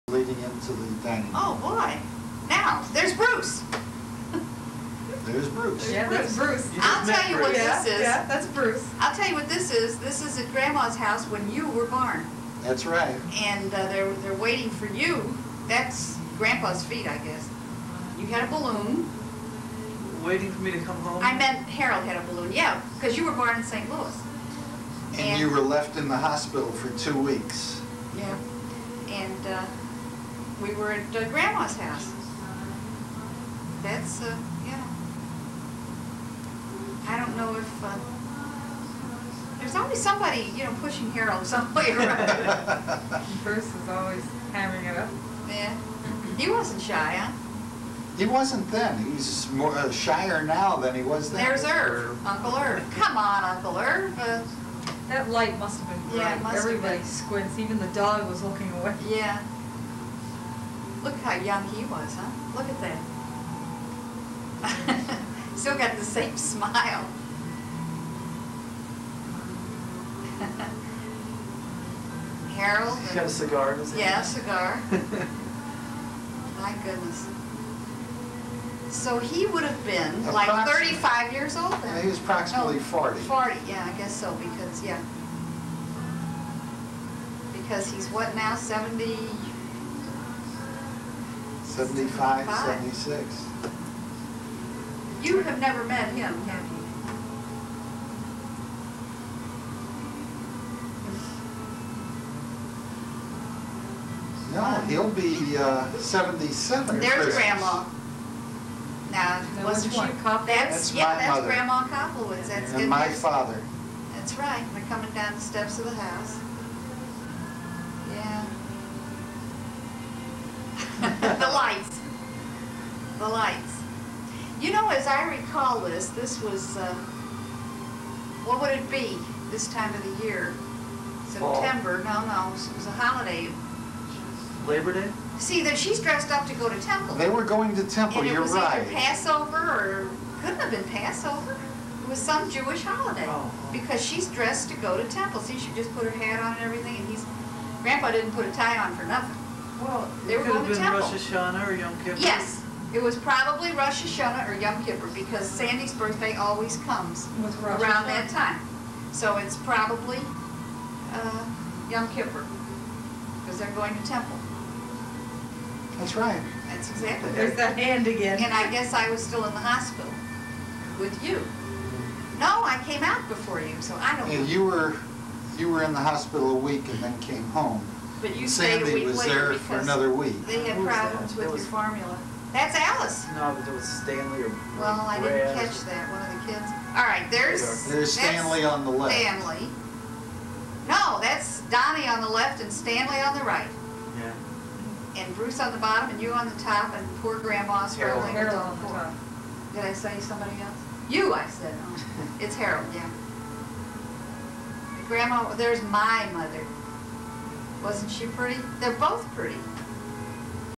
Koplowitz Family 1952
Koplowitz family 1952, at Harry and Rae's home in University City, St. Louis: Harold, Harry, Rae (Gurtman), Irv, Alice, Stanley and Donald Koplowitz; Bruce Goldstein; Rose (Zellinger, Bernard) Gurtman. Shot by Julius Koplowitz. Audio added in 1988 by Andrey, Julius, and Sandy Koplowitz.